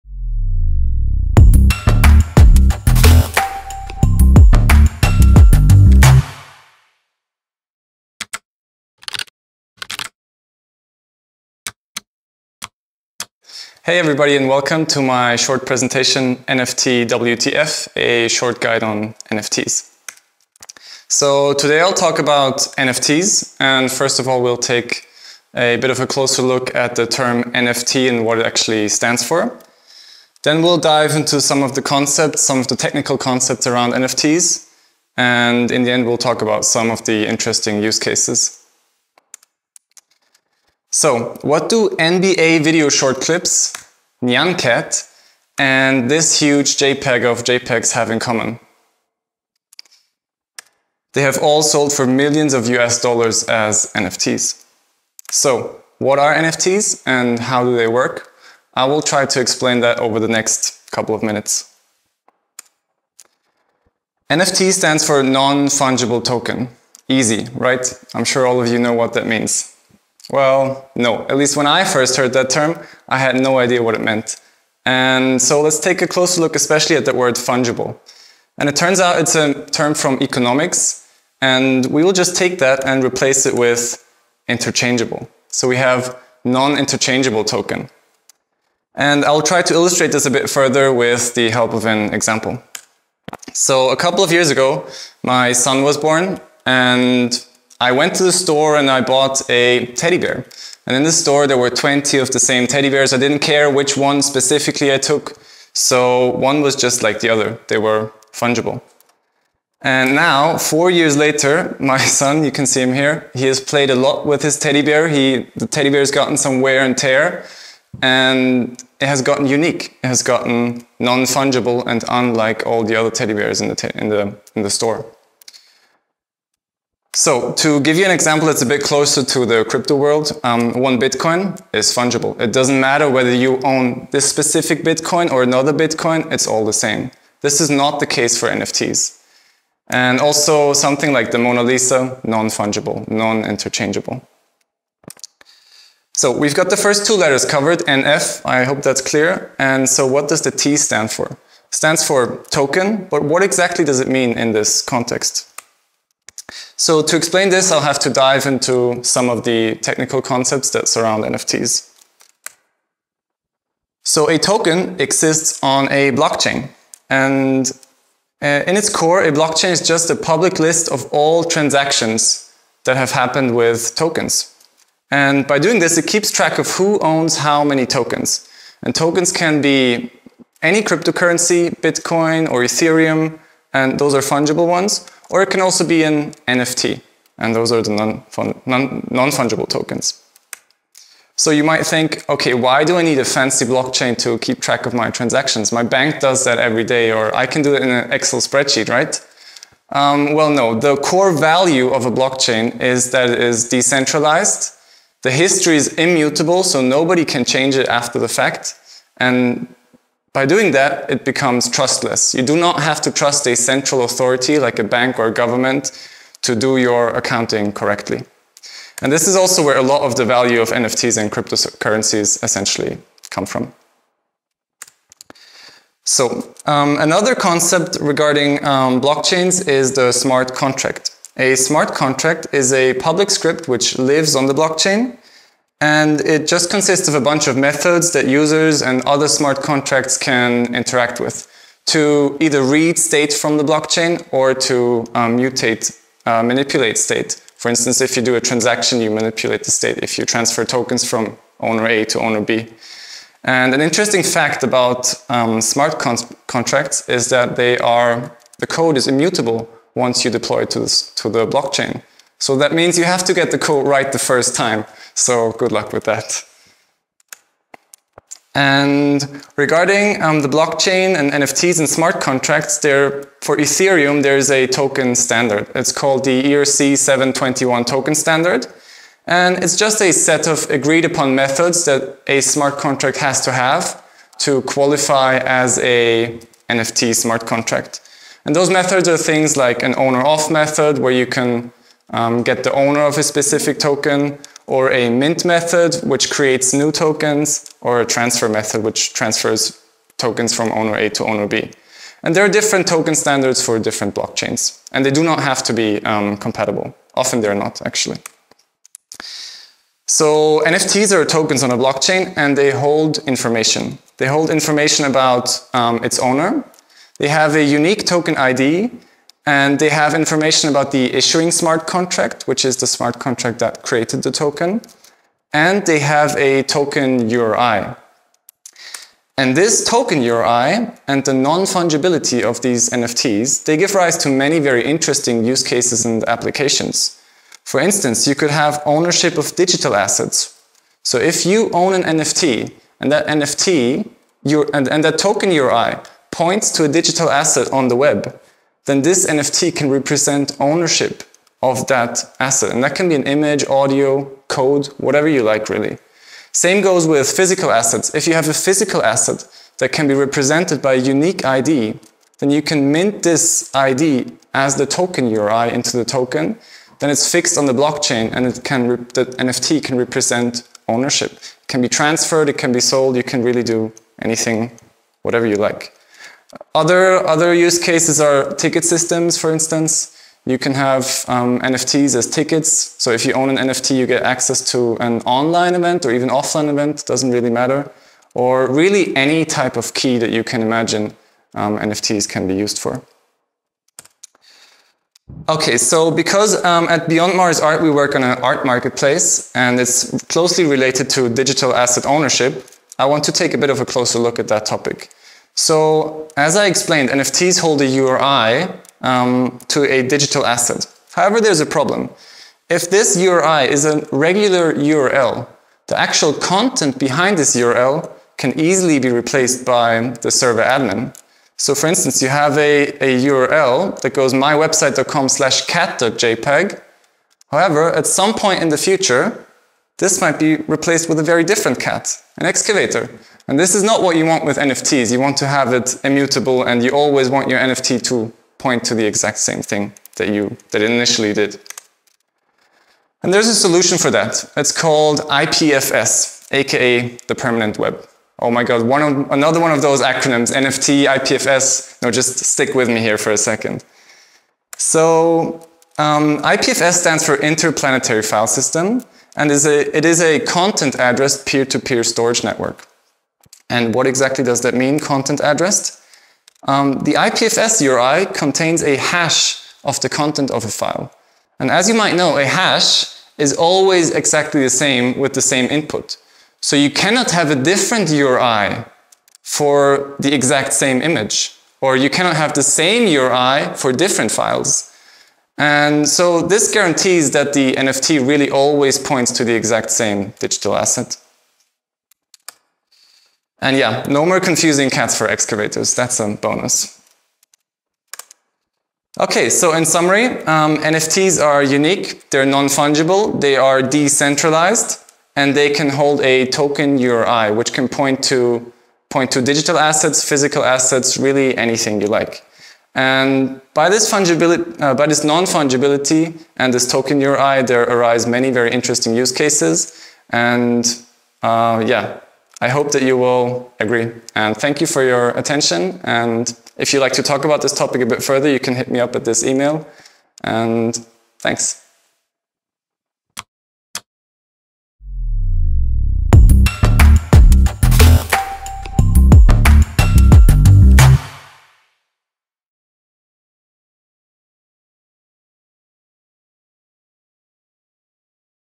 NFT - WTF?! – David Hauser @TechTalk Days 2021
David Hauser erörtert in seinem devjobs.at TechTalk was NFTs sind, welche Ideen dahinter stehen und auch warum "CAT" nicht gleich "CAT" ist.